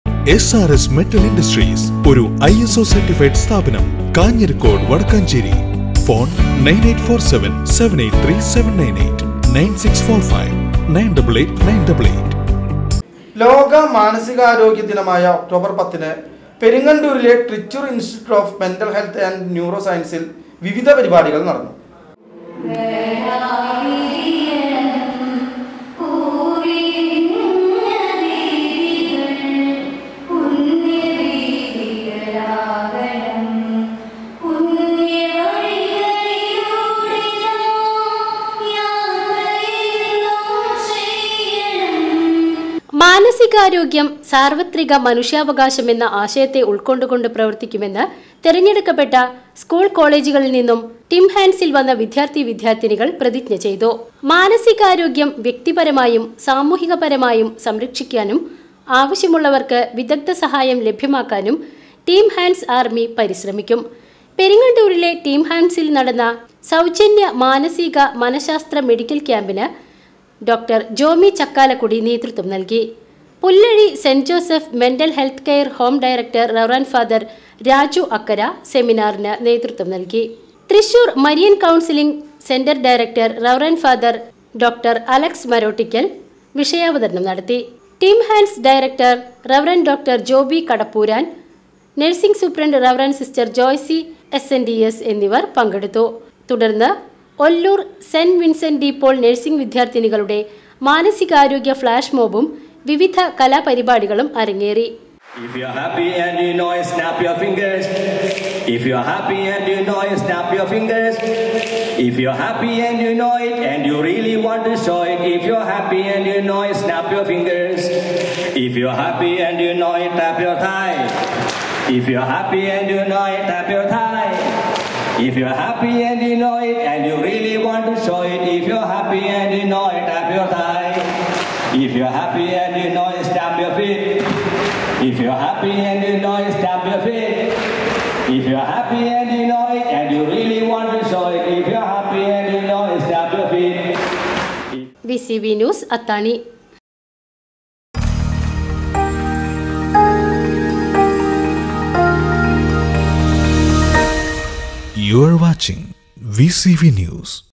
പെരിങ്ങണ്ടൂരിലെ ട്രിച്ചൂർ ഇൻസ്റ്റിറ്റ്യൂട്ട് ഓഫ്മെന്റൽ ഹെൽത്ത് & ന്യൂറോ സയൻസിൽ വിവിധപരിപാടികൾ നടന്നു
പെരിങ്ങണ്ടൂരിലെ ട്രിച്ചൂർ ഇൻസ്റ്റിറ്റ്യൂട്ട് ഓഫ് മെന്റൽ ഹെൽത്ത് & ന്യൂറോ സയൻസിൽ വിവിധ പരിപാടികൾ നടന്നു

VCV NEWS

VCV NEWS CHANNEL
Wadakkanchery, Thrissur